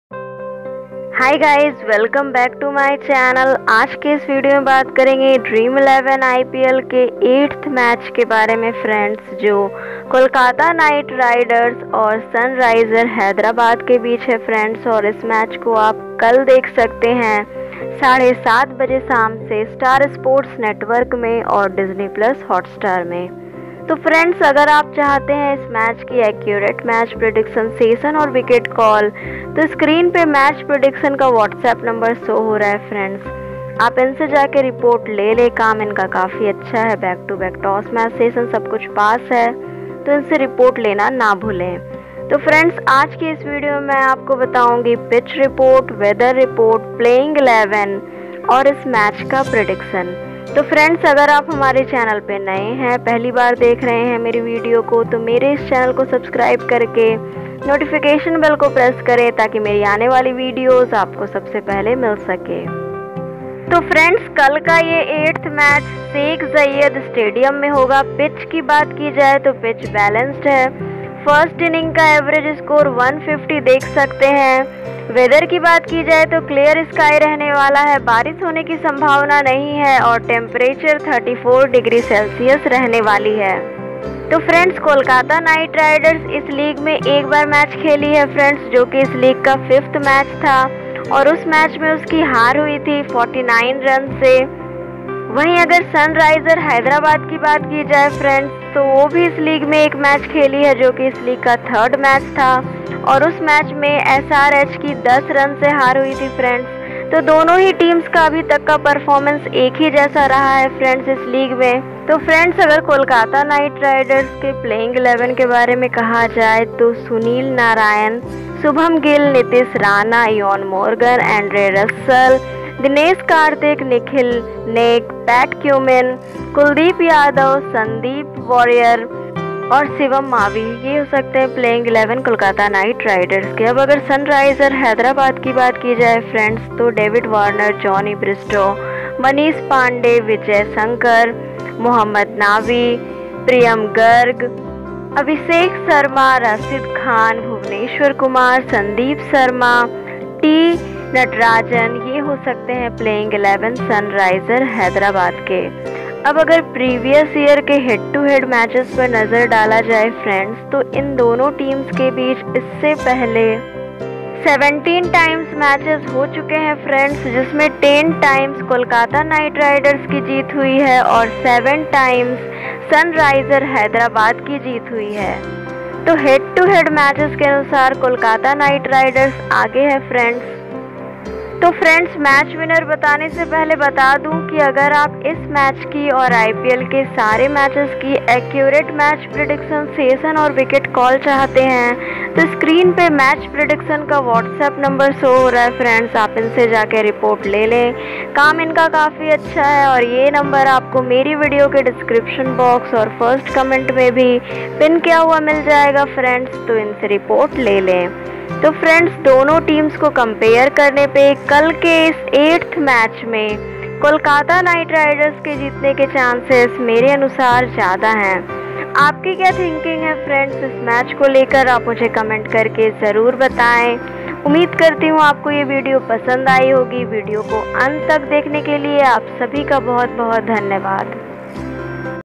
KOLKATA KNIGHT RIDERS VS SUNRISERS HYDERABAD 8TH MATCH PREDICTION
Kolkata Knight Riders vs Sunrisers Hyderabad: Probable Playing 11

Kolkata Knight Riders: Sunil Narine, Shubman Gill, Nitish Rana, Eoin Morgan, Andre Russell, Dinesh Karthik(w/c), Nikhil Naik, Pat Cummins, Kuldeep Yadav, Sandeep Warrier, Shivam Mavi

Sunrisers Hyderabad: David Warner(c), Jonny Bairstow(w), Manish Pandey, Vijay Shankar, Mohammad Nabi, Priyam Garg, Abhishek Sharma, Rashid Khan, Bhuvneshwar Kumar, Sandeep Sharma, T Natarajan